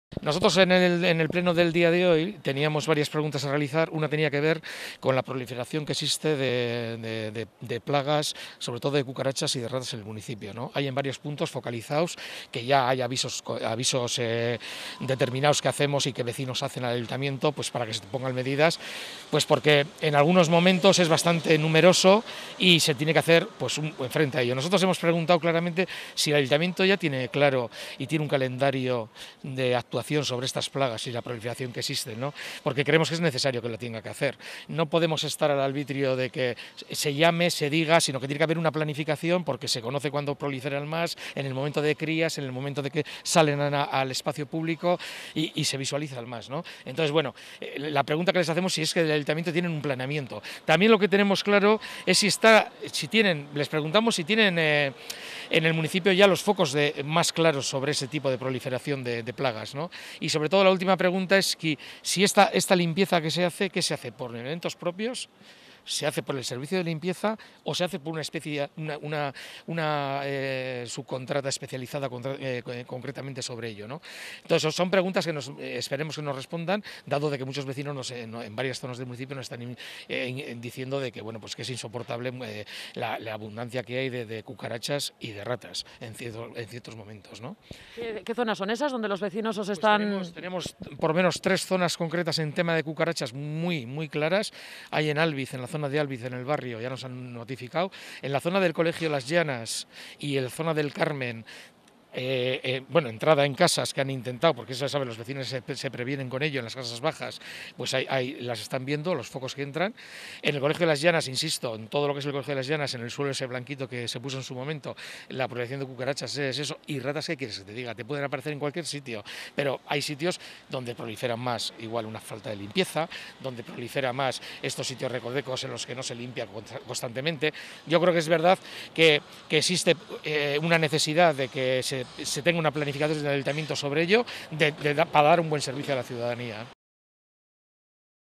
EHBildu alerta de la proliferación de ratas y cucarachas en Sestao
Son los propios vecinos  quienes les han hecho saber de determinados puntos donde este problema es  recurrente .

La coalición abertzale se pregunta si existe una planificación por parte del Ayuntamiento para adelantarse a posibles plagas de este tipo.